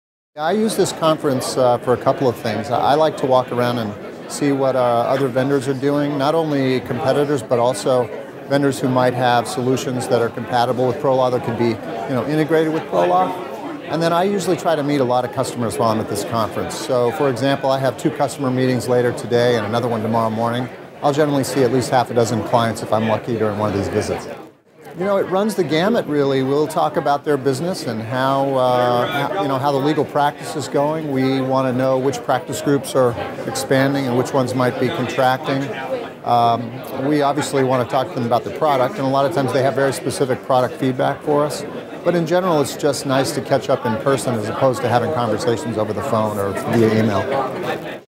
LegalTech 2009 - Matt DeVoll
Matt DeVoll, vice president, ProLaw. talks about what he hears from customers at LegalTech New York.